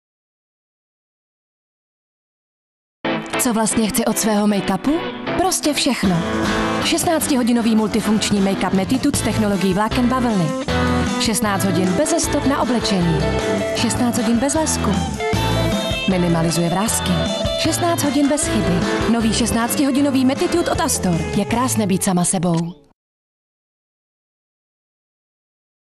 Astor Mattitude reklama
16hodinový make-up. Na Astoru mě fascinuje, jak propaguje heslo "je krásné být sama sebou" a přitom nabízí kosmetiku, která IMHO to "sama sebou" zapatlá. A klasicky, jak už to v dnešních reklamách bývá, dvacetileté modelky upozorňují na to, že propagovaný prostředek zakrývá vrásky. Kurňa, holky, jak to žijete, že máte ve dvaceti vrásky??